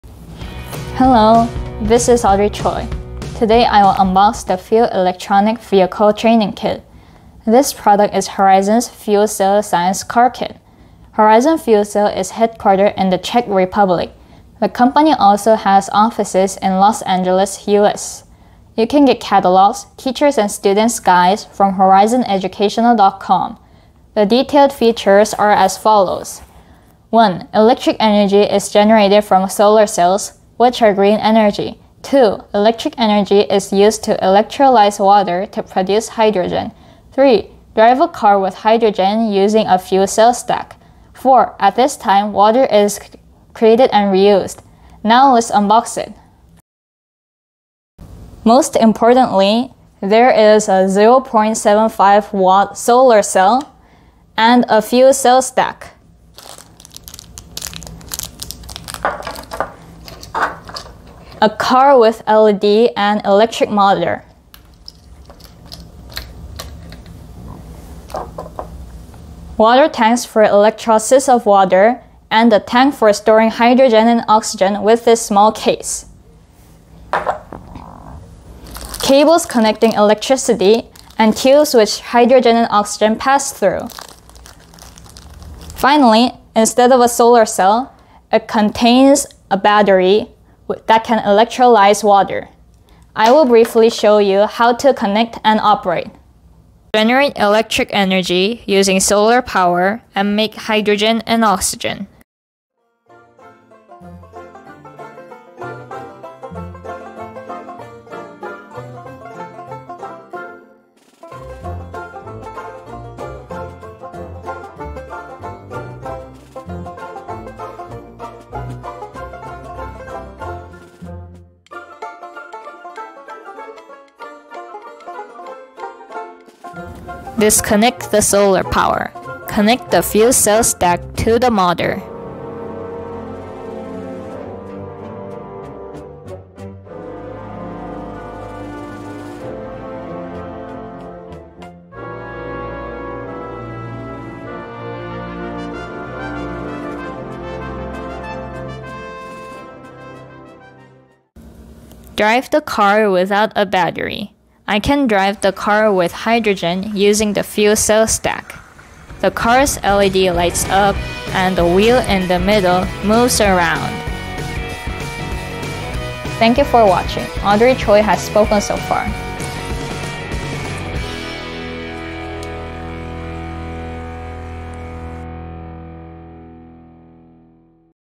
Hydrogen Fuel cell for Kids, Unbox the fuel cell training kit ( Horizon's)
Hydrogen Fuel cell for Kids
I unboxed the fuel electronic vehicle training kit(Top view, Short version)

This product is Horizon's fuel cell science car kit.
1. Electric energy is generated from solar cells, which are green energy.
2. Electric energy is used to electrolyze water to produce hydrogen.
3. Drive a car with hydrogen using a fuel cell stack.
4. At this time, water is created and reused.

Fuel cell car with solar power and hydrogen.(as Hyundai Nexo and Toyota Mirai)

Audrey Choi